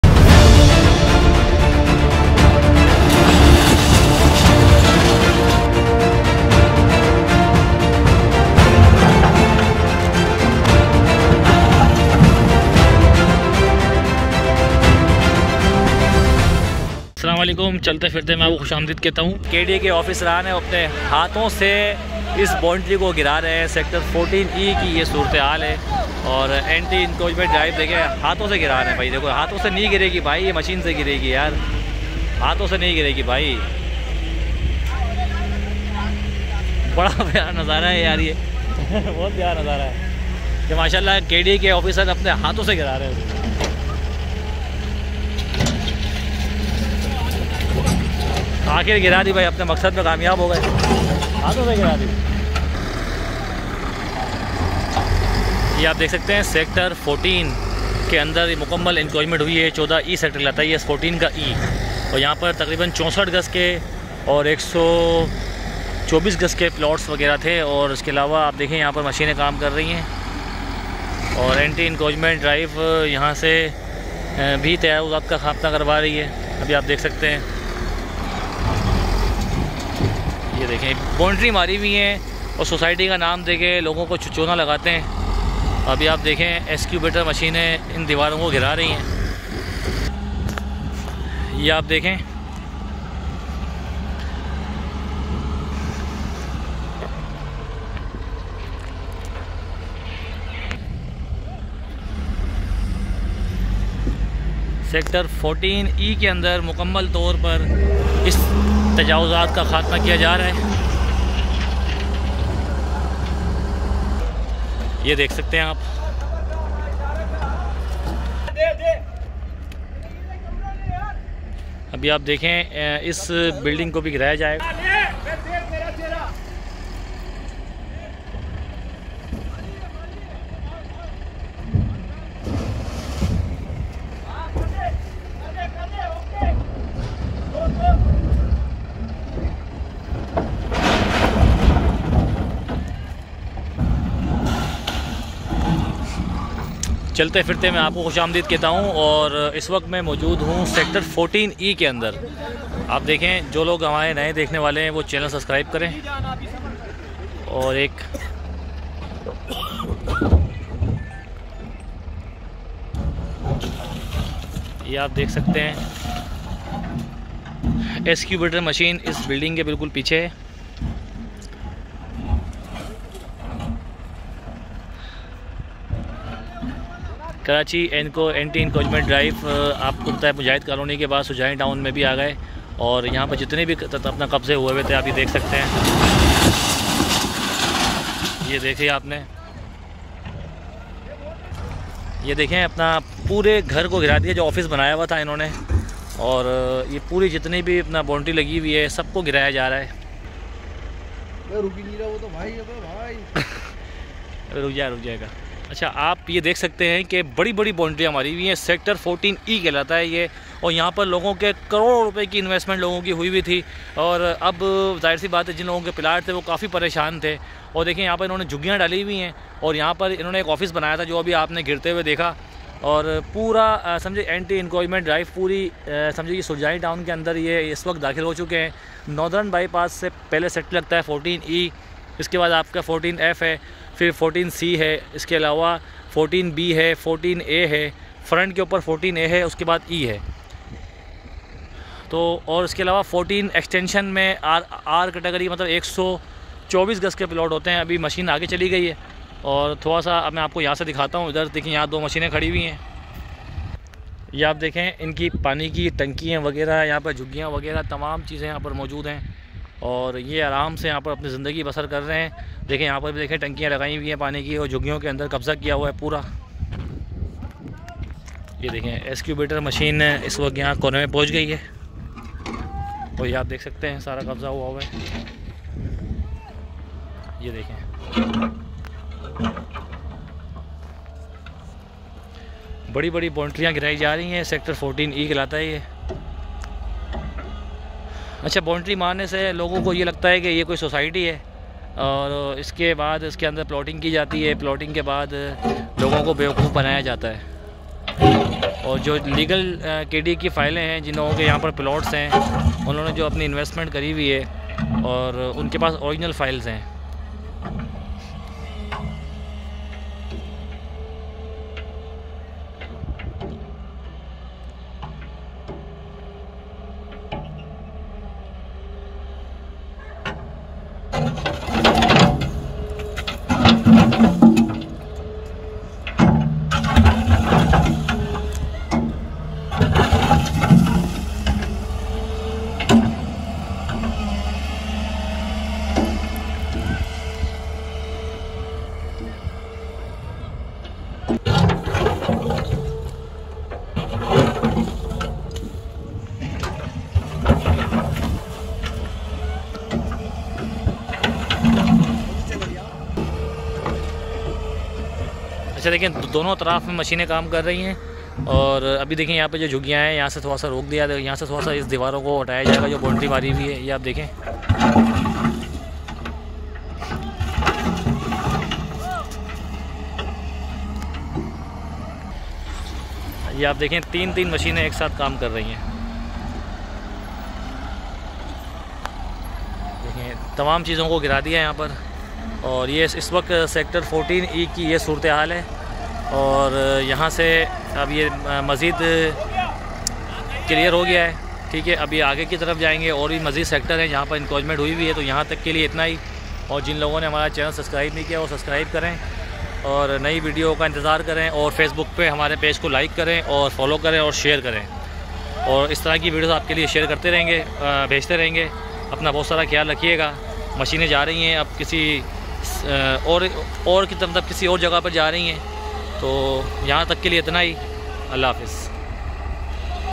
Anti Encroachment operation at Surjani Town | Surjani Town House Demolished
Anti Encroachment operation at Surjani Town
Surjani Town House Demolished

 YouTube 👉 👉 chaltayphirtay
Follow   👉 👉 chaltay_phirtay2020